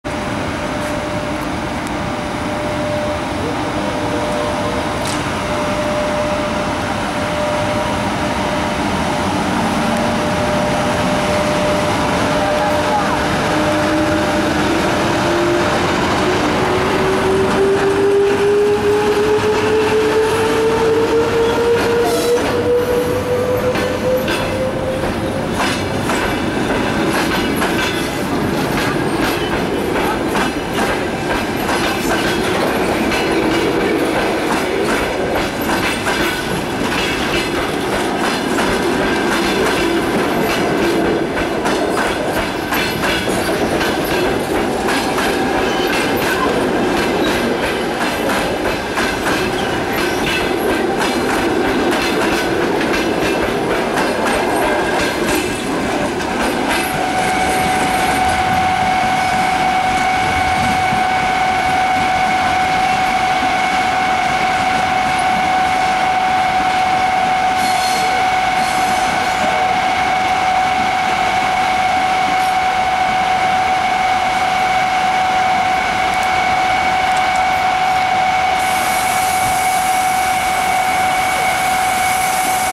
ЭП1М-658 с фирменным поездом отправляется со ст.Балезино
Электровоз ЭП1М-658 с фирменным поездом "Кама"  №007Е сообщением Пермь-Москва  отправляется со ст.Балезино
Россия/Удмуртия
15.07.18